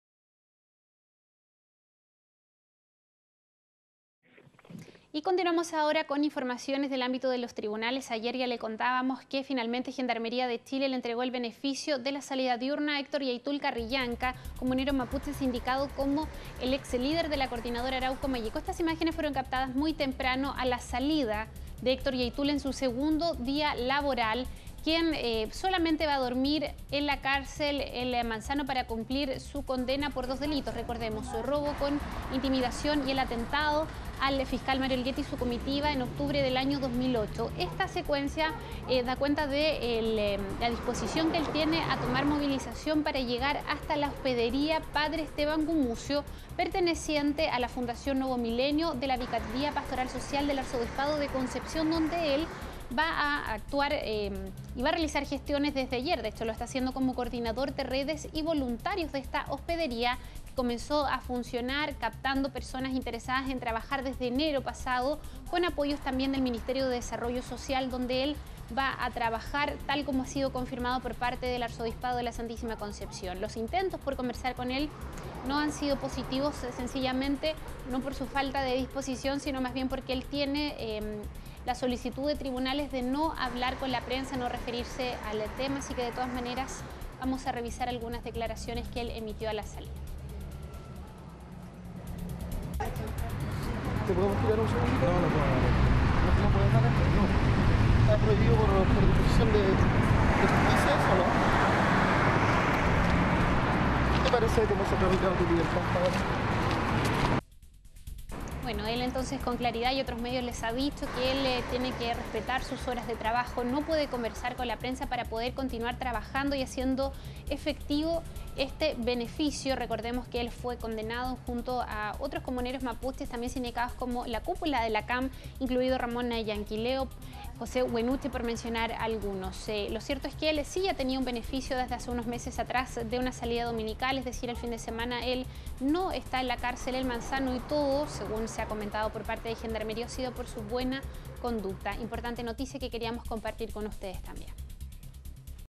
Comunero Héctor Llaitul cumplió primer día de libertad diurna tras dictamen judicial
El líder mapuche se integró a trabajar en una institución ligada al Arzobispado de Concepción.